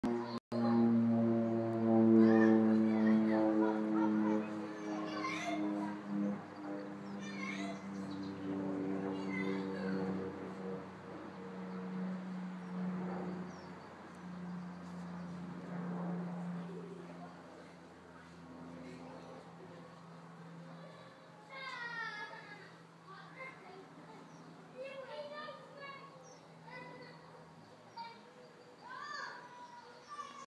Flightline Flight 7751(Unknown to BHX)
Flightline Flight 7751 from an unknown to Birmingham Airport. The aircraft is Fairchild Swaeringen SA-226AT Merlin IVA (reg: EC-JCV) is 40 years old and has been operating for Flightline since 2005.